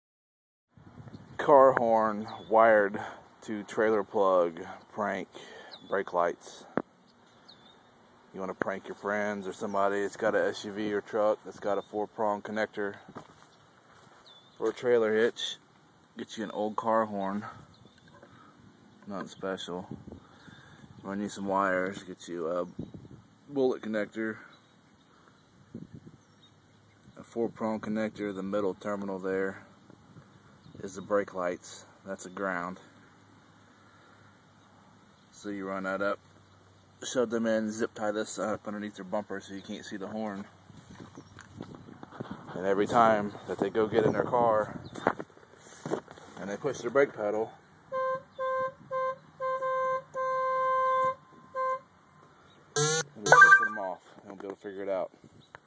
car horn wired to break lights easy funny prank
how to prank someone with old car horn wired to break lights trailer plug easy to set up and remove  just plug it into your victoms 4 prong trailor light connector and zip tie it underneth  truck no harm done easy funny prank
you need old car horn some wire bullet connectors zip tie or way to tie it up out of the way and someone to prank that has a trailer plug on their car truck van suv